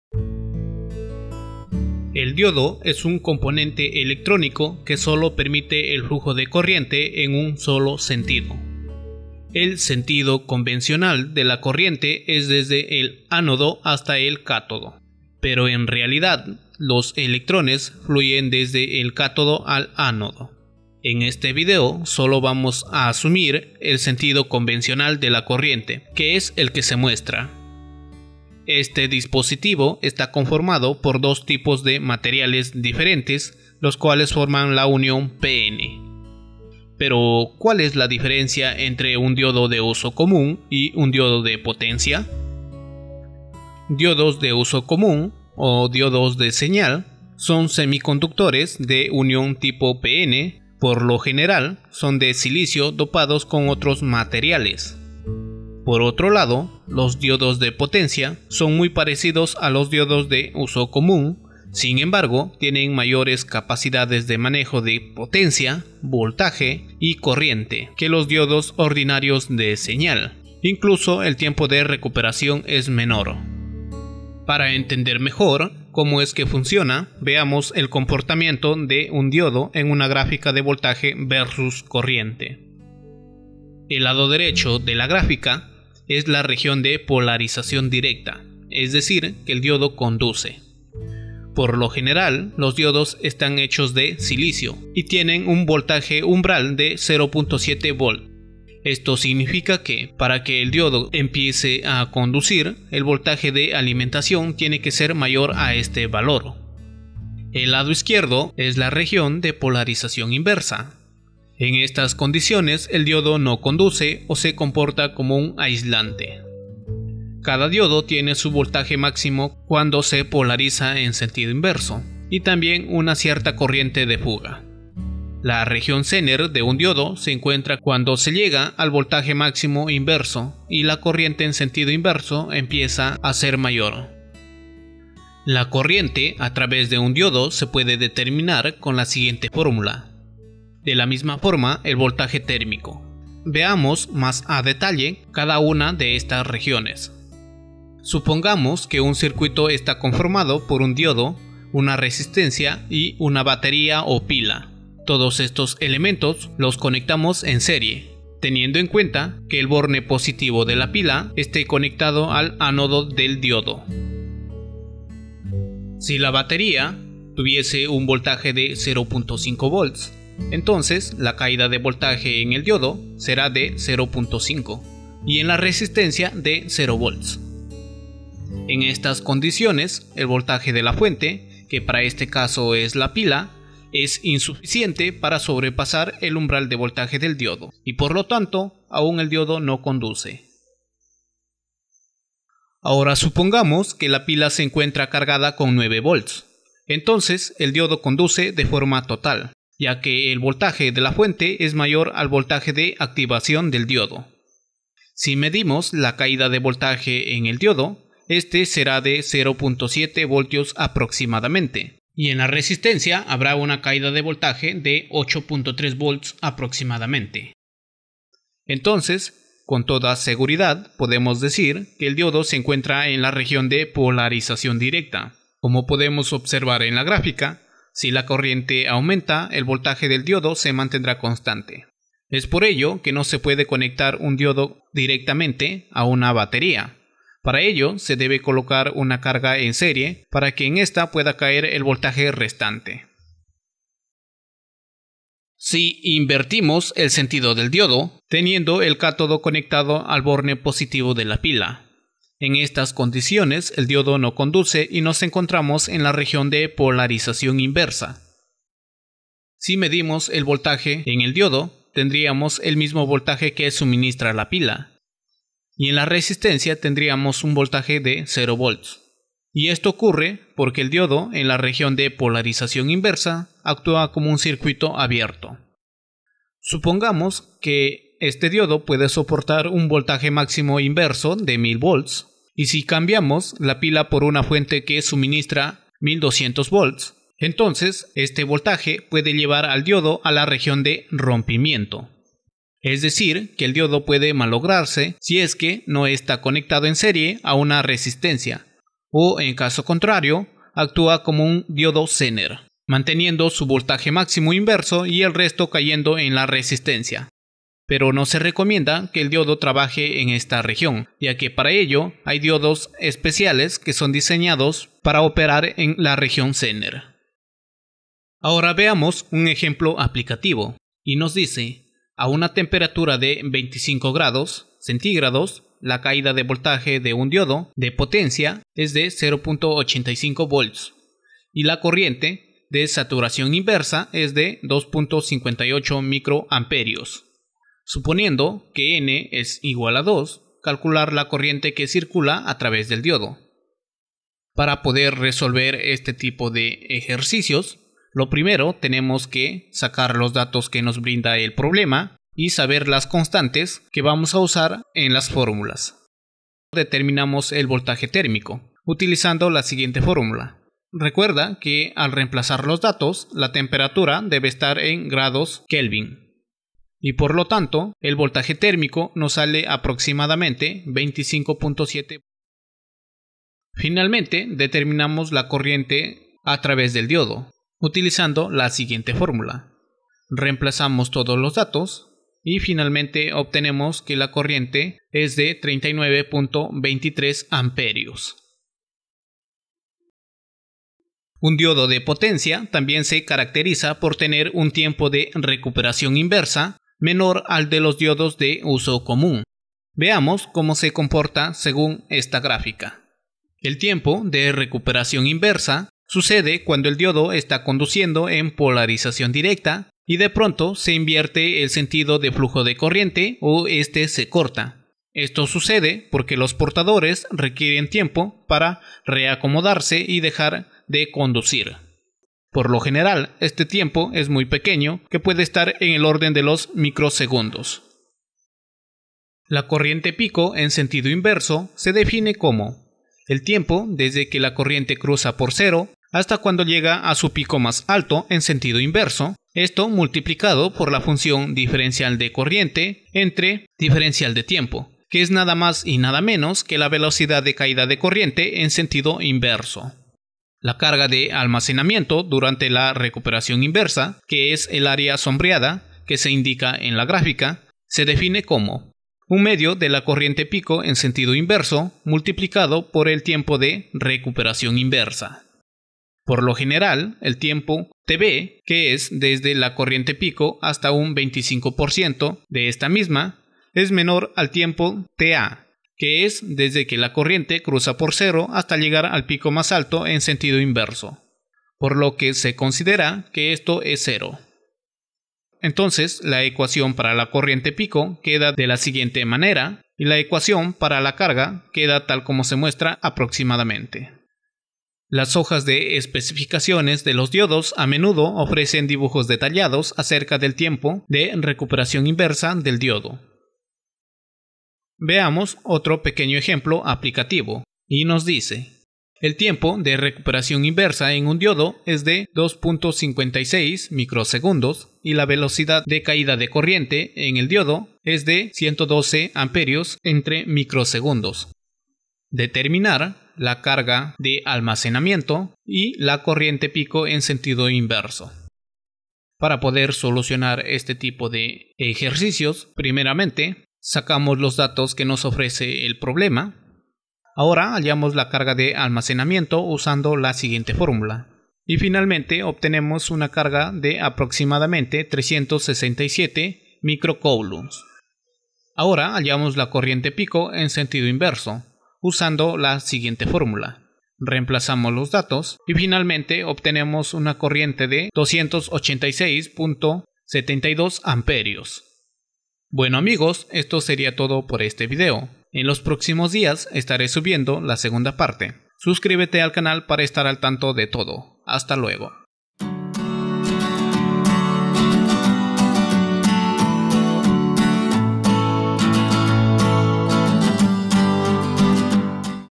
DIODOS DE POTENCIA | SEMICONDUCTORES
En este video, se explica una parte sobre los diodos de potencia. Como determinar su corriente y carga de almacenamiento.

B. A. Y.